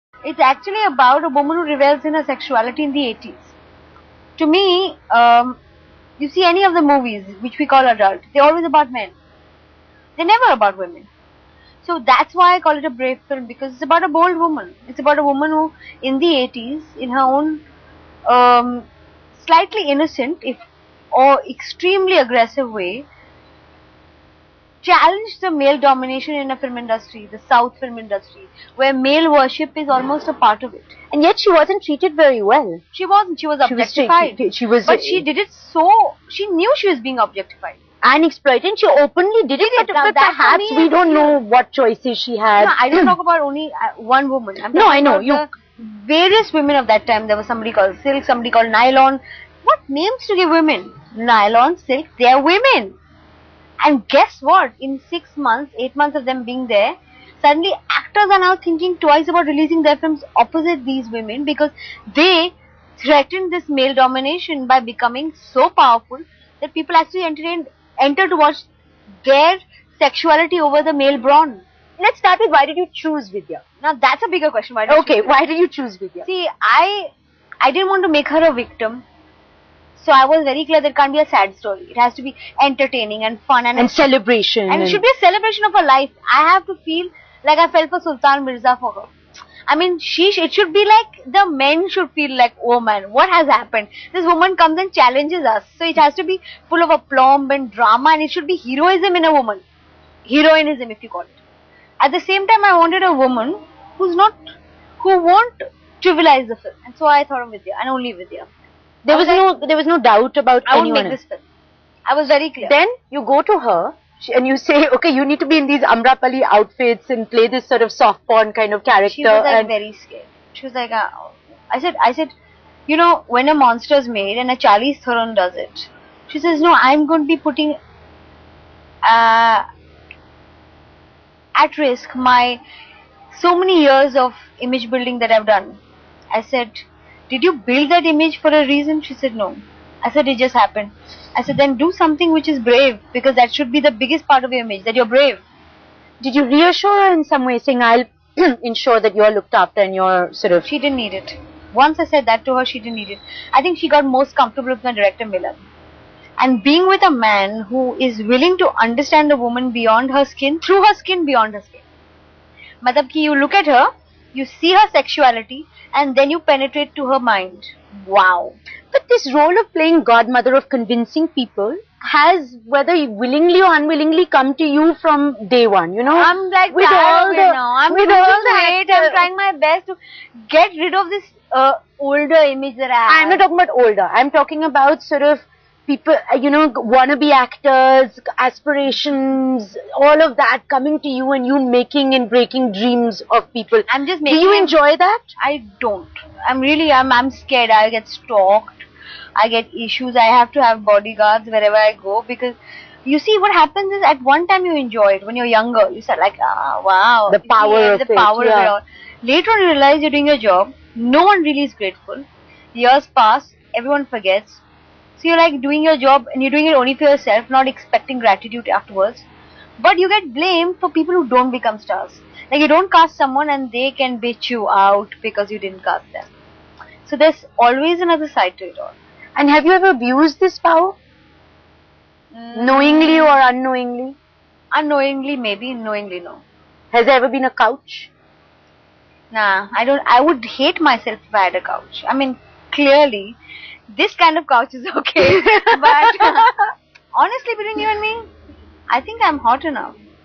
Destiny has changed me: Ekta | Part 3
Producer-director Ekta Kapoor says she gets ideas for her tv shows from people.